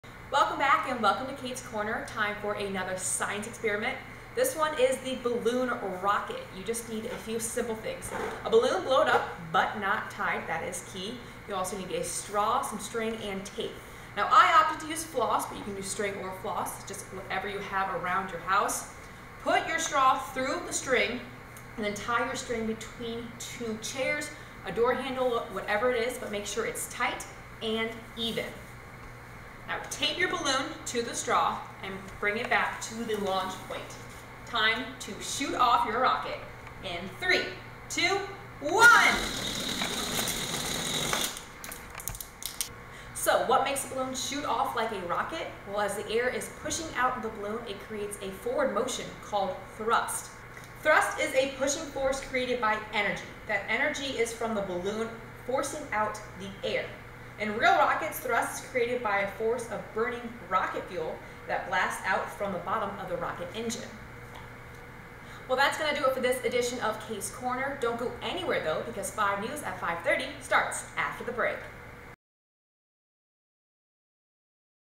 Kate's Corner: Balloon Rocket
In this Kate's Corner, we are going to make a balloon rocket. All you need is a few simple things. First is a balloon blown up but not tied. You'll also need a straw, some tape, and string.